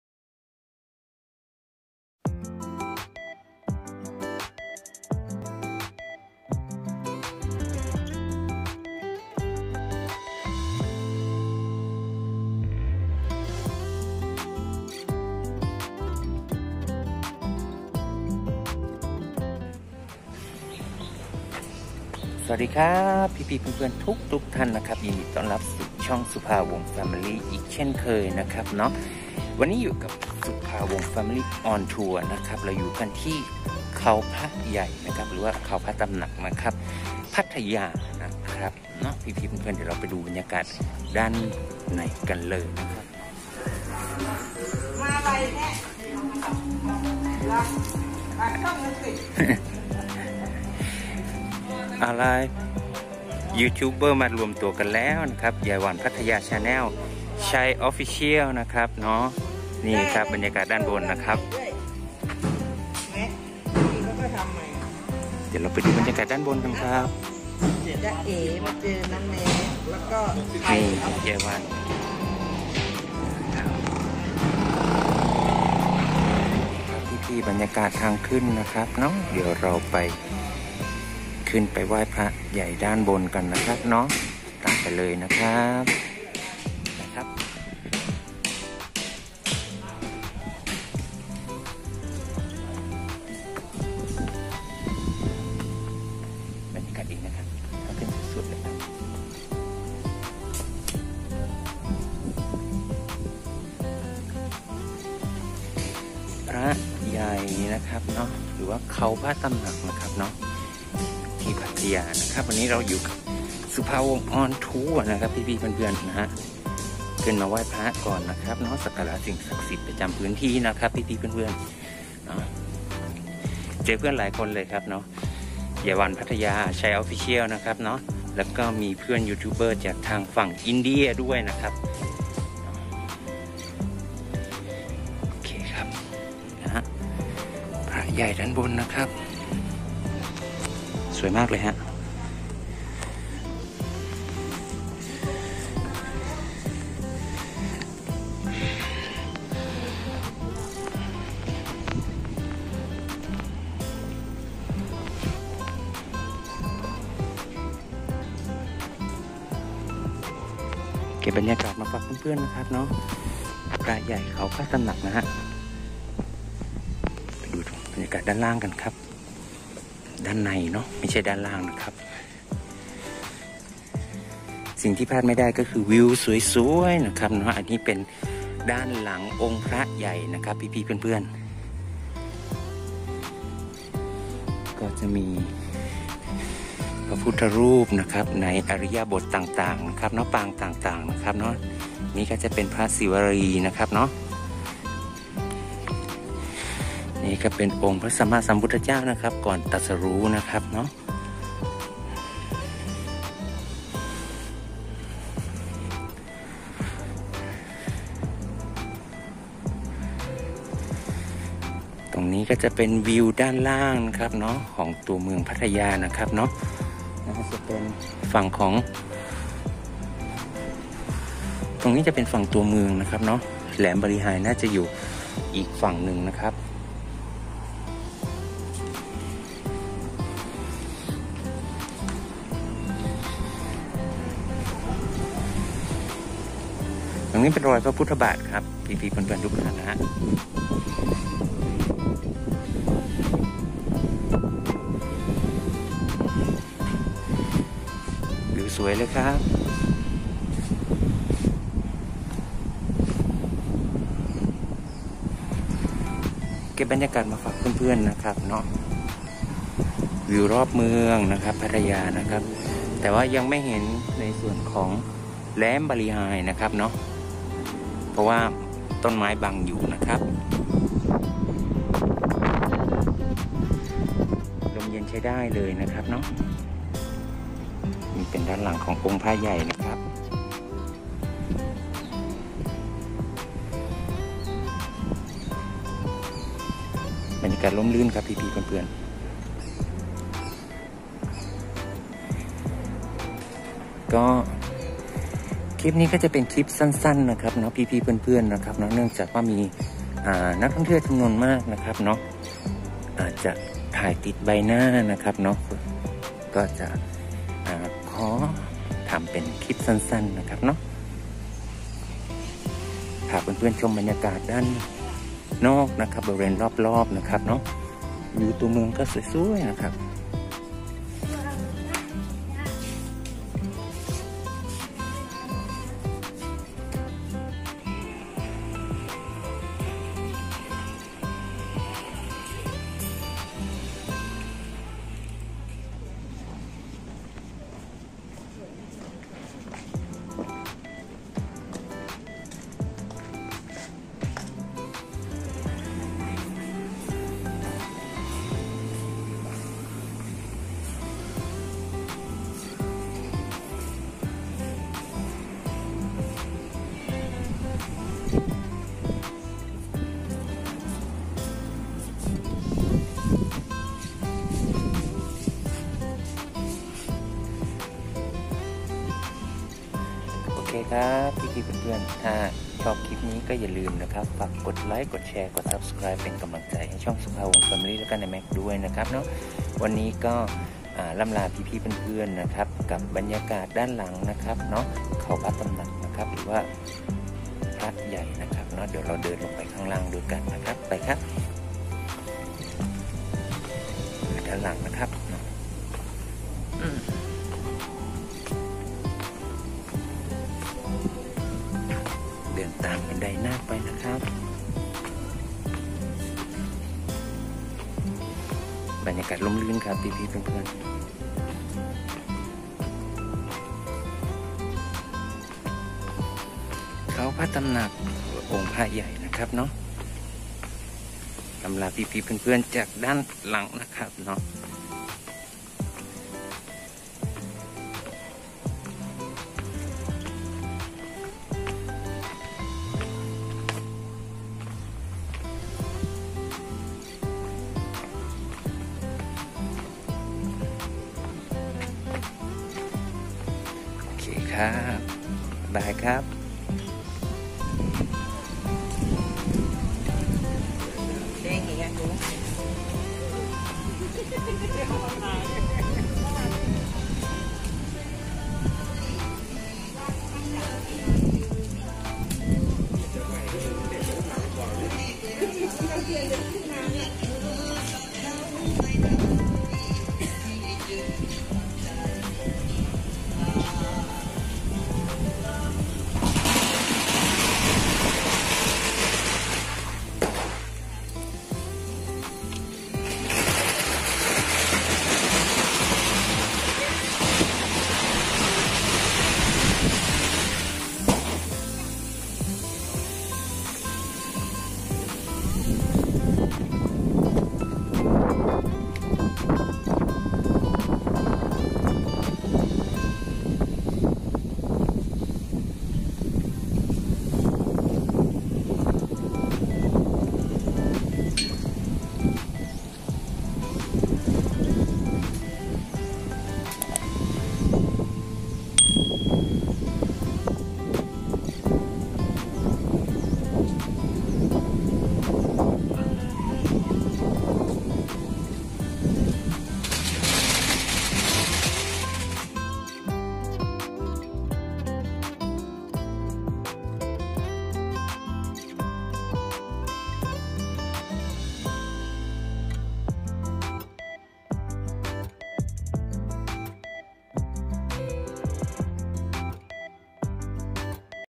วัดพระใหญ่ เขาพระตำหนัก ชมวิวเมืองพัทยา จุดชมวิว สทร 5 พัทยา
สวัสดีครับพี่ ๆ เพื่อน ๆ ทุกท่าน มาพบเจอกันอีกเช่นเคย กับนายแม็คและช่อง @Vinz41236  วันนี้นายแม็ค พาเพื่อน ๆ ทุกท่าน มาเที่ยวเมืองพัทยาครับ ในคลิปนี้ไม่ได้มีแต่นายแม็คนะครับ ยังมีเพื่อน ๆ ยูทูปเบอร์ อีกหลายท่าน @Manna55 @Thailand Diaries @wandee @ยายวัน พัทยา ชาแนล ก่อนที่เราจะไปเที่ยวบริเวณรอบเมืองก็ต้อง สักการะสิ่งศักดิ์สิทธิ์ ประจำพื้นที่ก่อนนะครับ วันนี้นายแม็ค พาเพื่อน ๆ ขึ้นเขาพระตำหนัก กันครับ ไหว้พระใหญ่บนเขา กันครับ.. ก่อนไปรับชมคลิป ขอคนละ 1 ไลค์และ Subscribe เป็นกำลังใจให้กับนายแม็คและทางช่อง @Vinz41236 ด้วยนะครับ

พร้อมเพย์ : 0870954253 (คนละ 1 บาทแอดมินก็ดีใจและนับเป็นพระคุณอย่างยิ่ง)

ขอบคุณทุกท่านสำหรับการติดตามรับชม